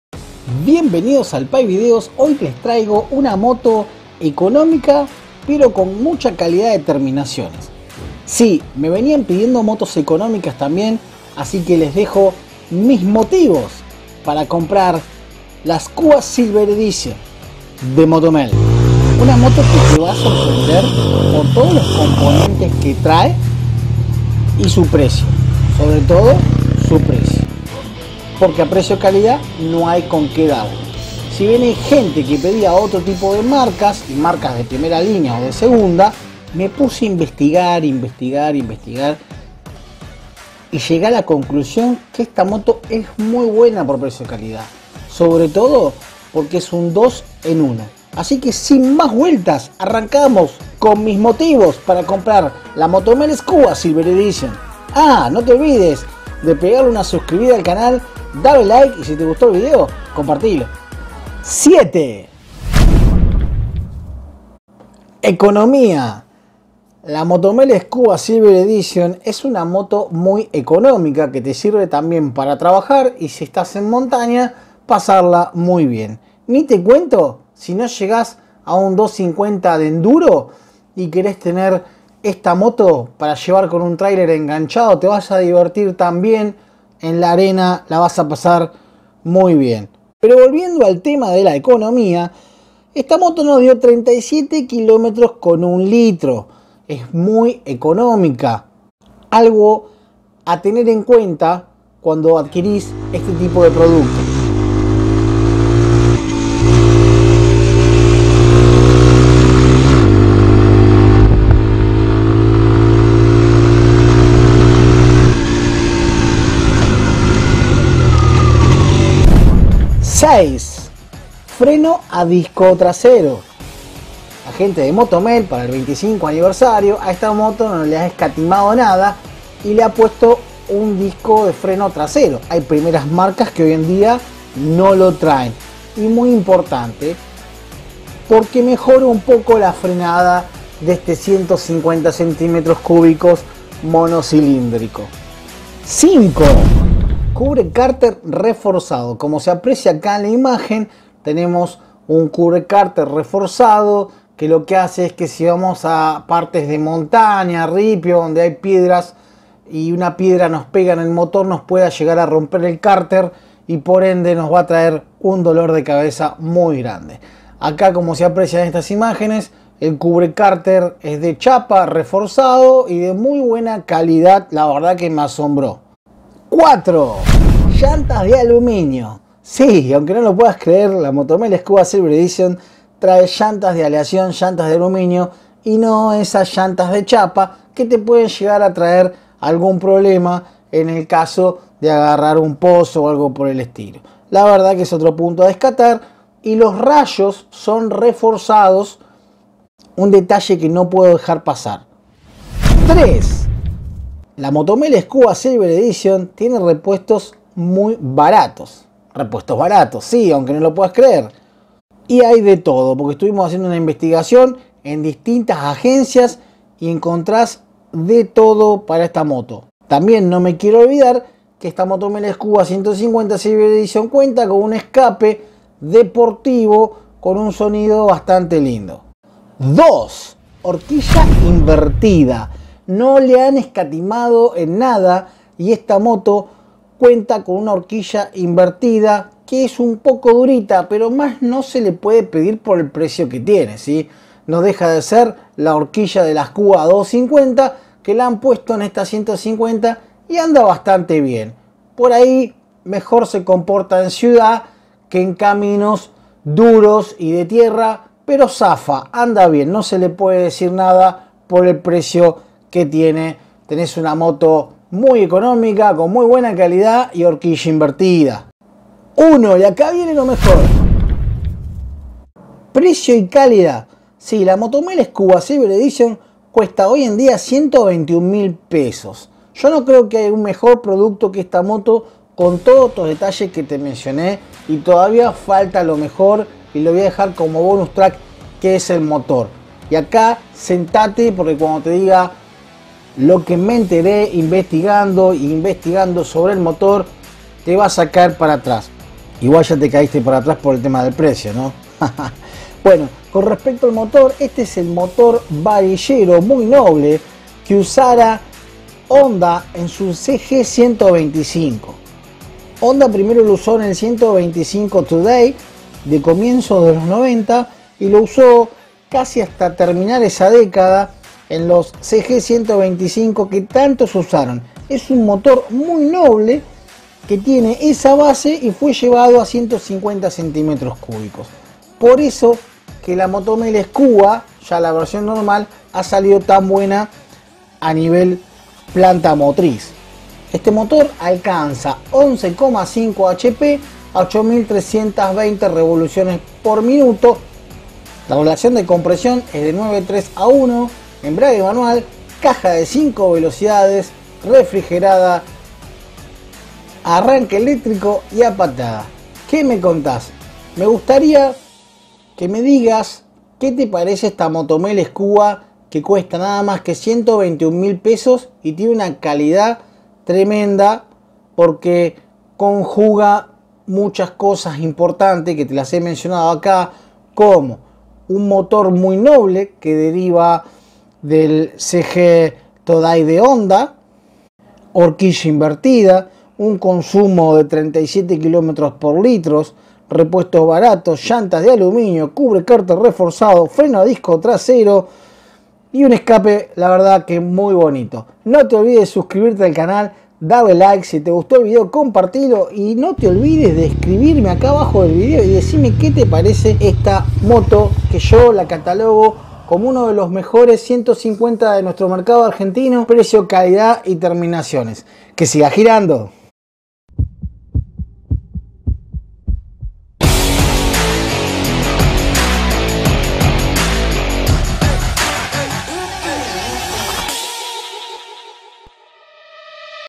Mis motivos para comprar la Motomel skua 150 silver edition v6
En esta oportunidad les traigo la con mejor prestaciones ,precio calidad del mercado a mi entender
Conviértete en miembro de este canal para disfrutar de ventajas:

Podes hacerte miembro vip del canal donde esta el boton unirte
 Grupò de facebook - el pai videos